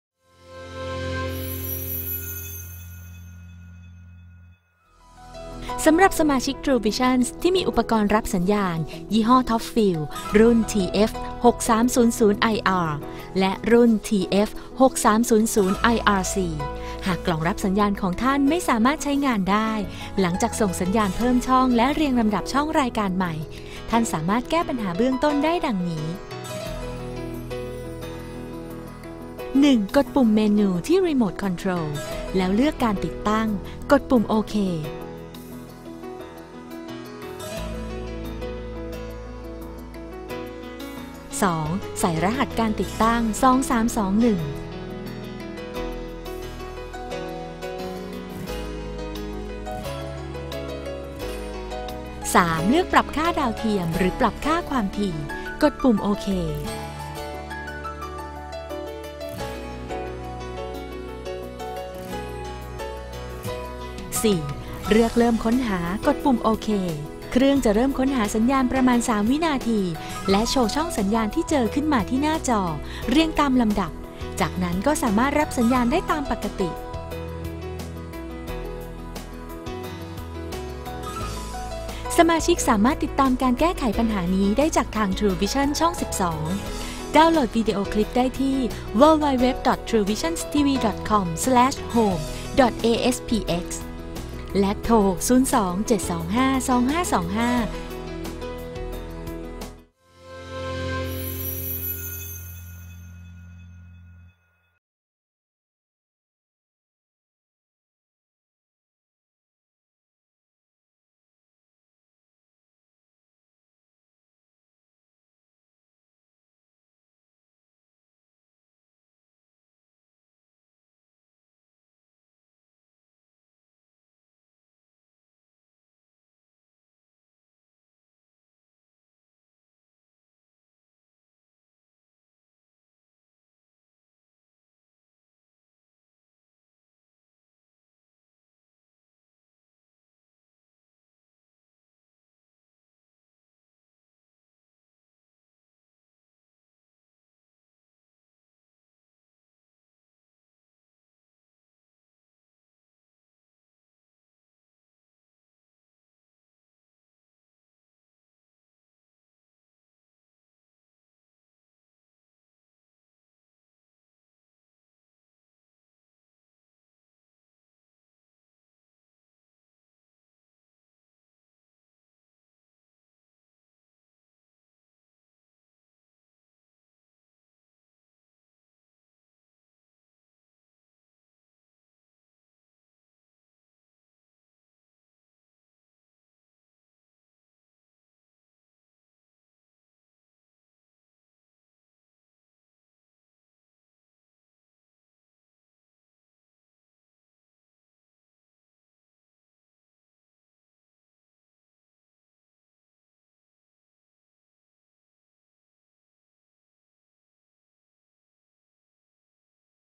คู่มือการแก้ไขกล่องเครื่องรับสัญญาณรุ่น TOPFIELD รุ่น TF6300IR และ TF6300IRC
กรณีท่านสมาชิกไม่สามารถรับชมช่องรายการต่างๆ ของทรูวิชั่นส์ได้ ท่านสมาชิกสามารถแก้ไขปัญหาการรับชมเบื้องต้นได้ด้วยตนเอง
โดยถอดปลั๊กเครื่องรับสัญญาณออกแล้วเสียบใหม่ หากยังไม่สามารถรับชมได้ท่านสมาชิกสามารถแก้ไขปัญหาได้ตาม vdo นี้

TOPFIELD (TF6300IR) and (TF6300IRC)
If you are encountering reception problems, as primary problem-solving: Simple switch off and unplug your receiver and after a few minute, plug it in again and turn it on. Where this primary problem solving does not rectify the problem, please follow the menu step for each model each model as per the following;